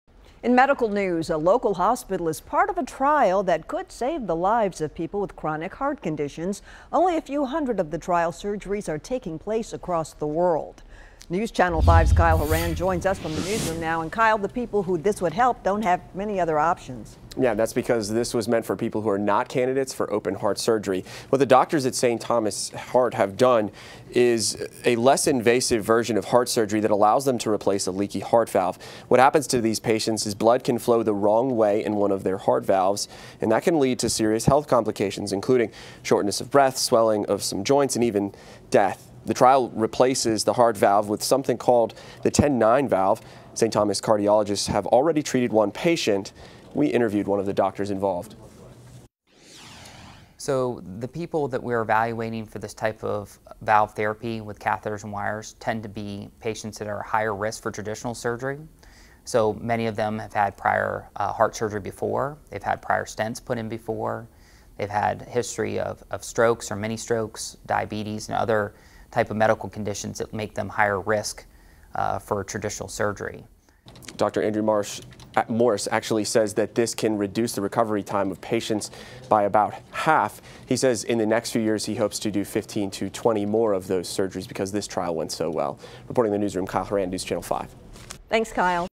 St. Thomas doctors perform cutting edge heart valve procedure
The doctors at St. Thomas Heart have performed an experimental and potentially life changing surgery that could change the way some people with heart conditions are treated.